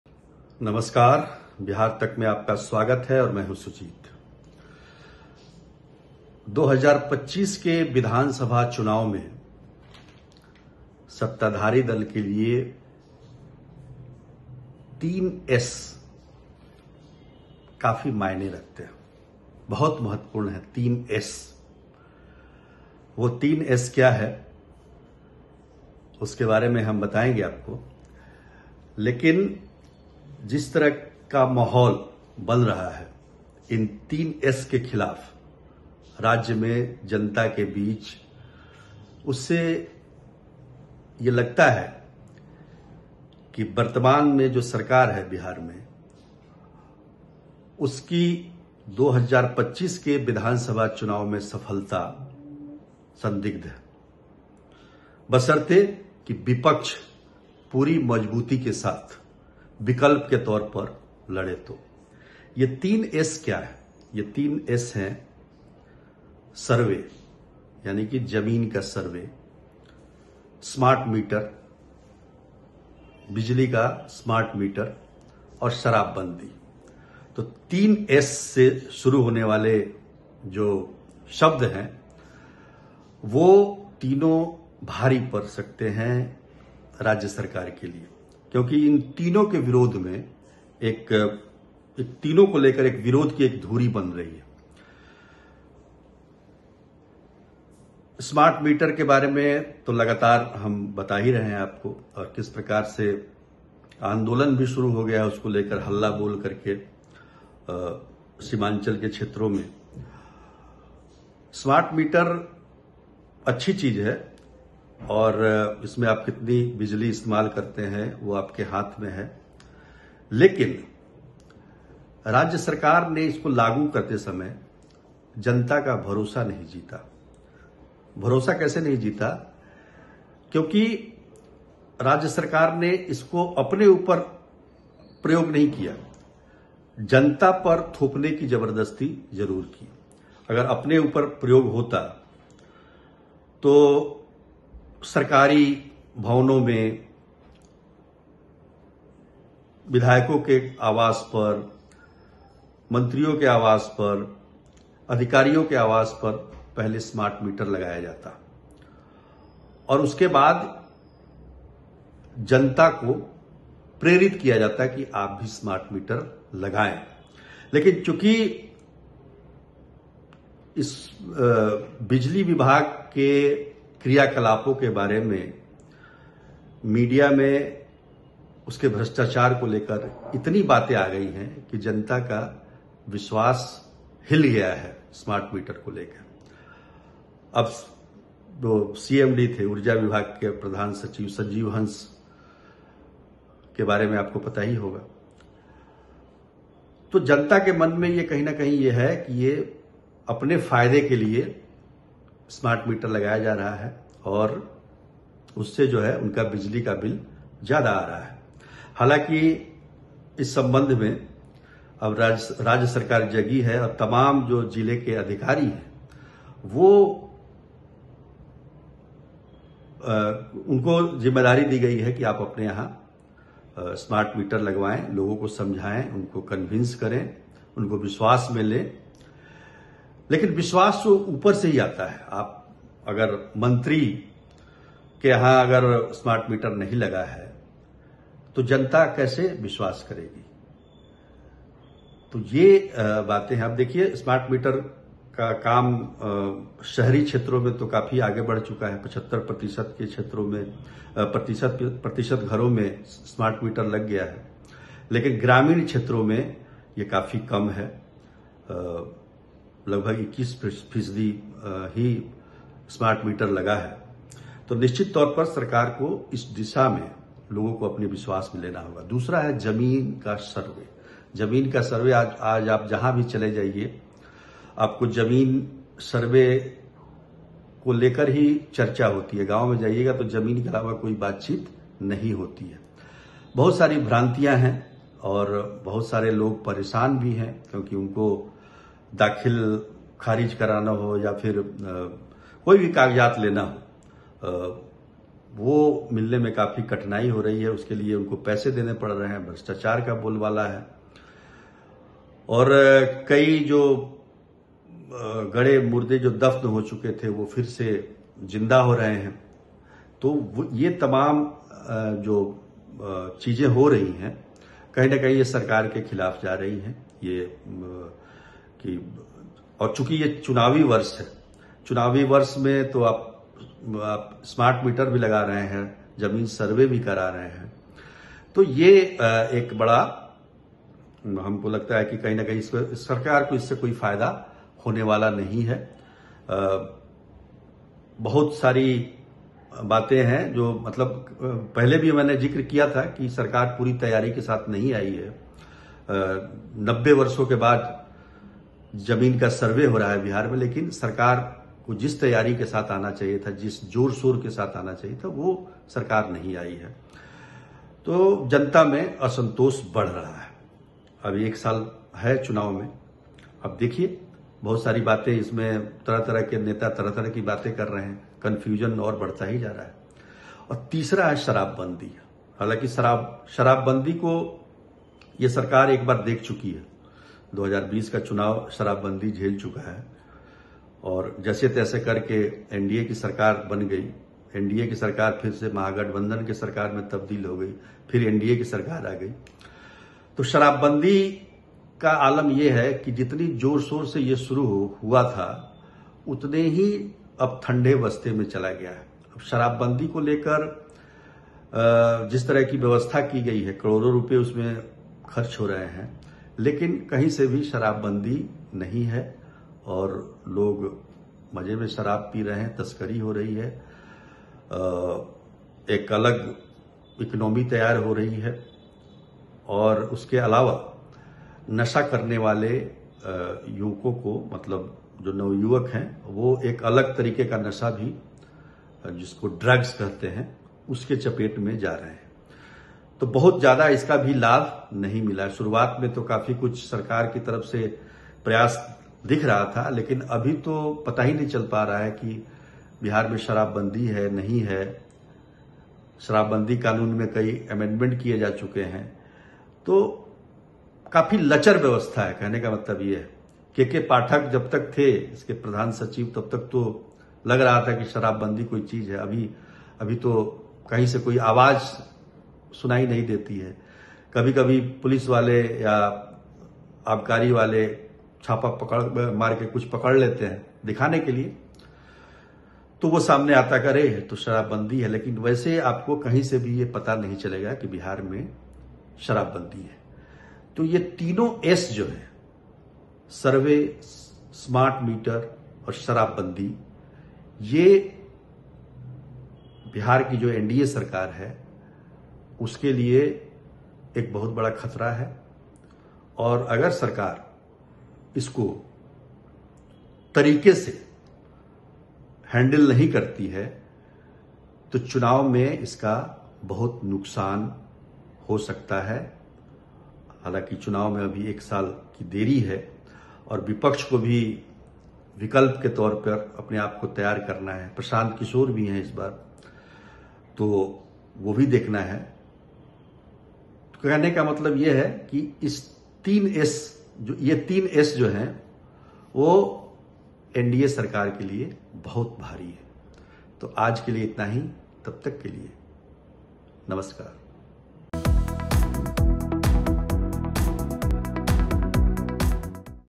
Explainer: Nitish Kumar के सिरदर्द बनेगा 'तीन S'? क्या चुनाव में डूब जाएगी लुटिया | Bihar Tak
नीतीश कुमार की सरकार के लिए क्या आने वाले दिनों में जमीन सर्वे, बिजली का स्मार्ट मीटर और शराबबंदी परेशानी का सबब बनने वाला है? क्या ये तीन मुद्दे विपक्ष के लिए चुनाव में सबसे बड़े मुद्दे होंगे जहां सरकार को कई चुनौतियों का सामना करना पड़ रहा है? इसी मुद्दे पर देखिए